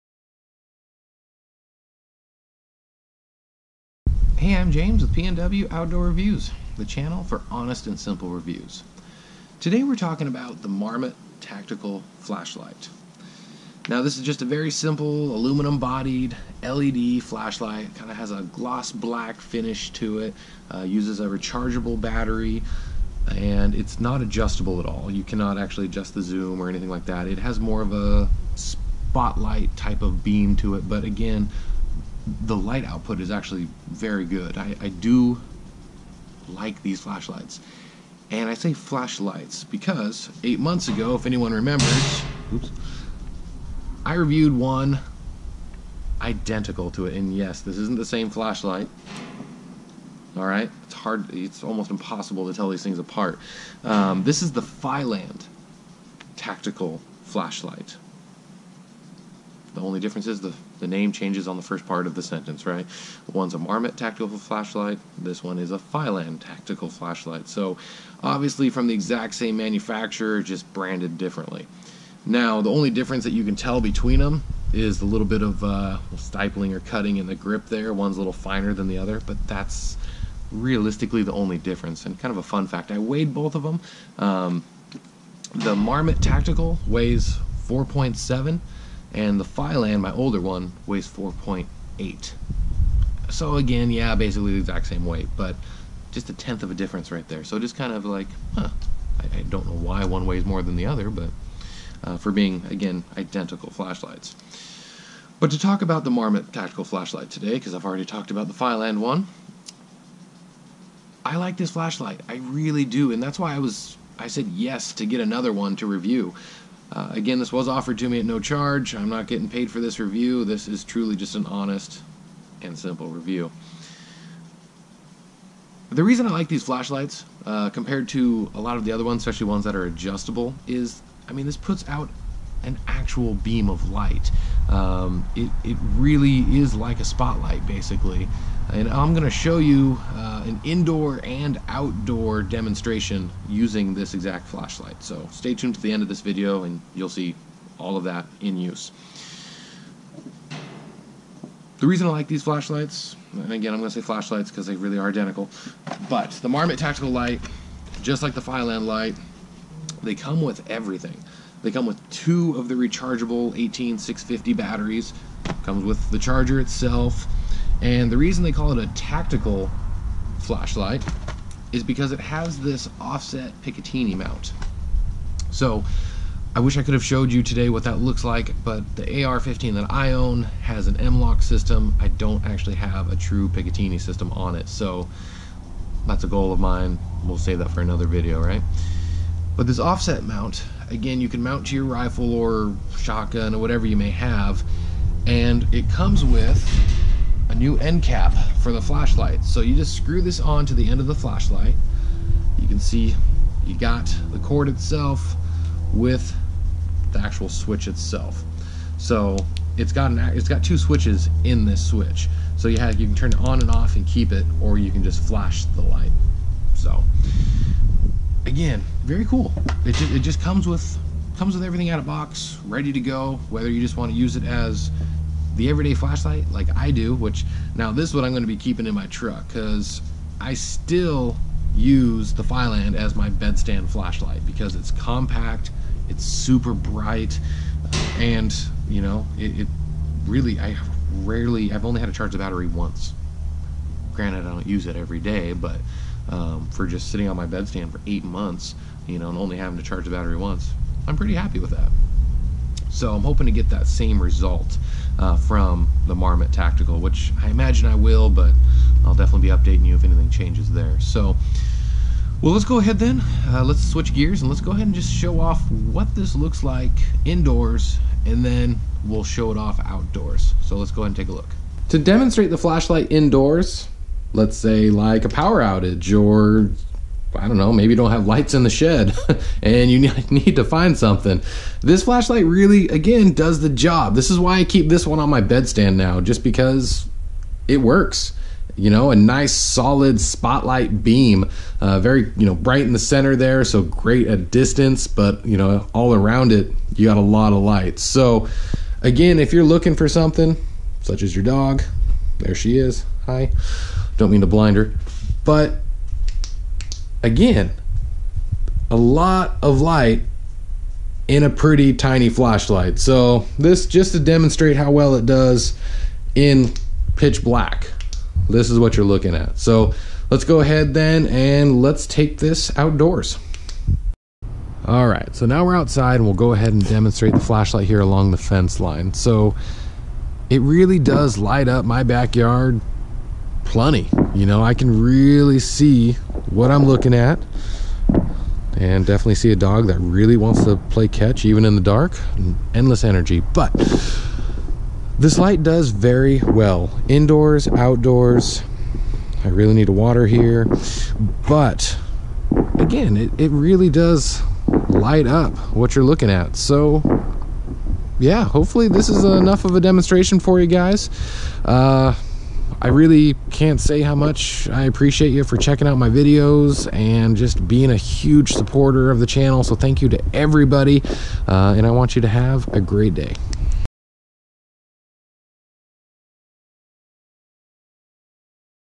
Marmot Tactical Flashlight Review w/ Demonstration
A budget friendly LED flashlight that simply just works on or off your rifle/shotgun.

* 1 negative about this light that I forgot to mention in this video is the fact it's not waterproof. In the rain this flashlight will probably be just fine but drop in the water then it will come in around the lens.

Can be found on amazon for purchase.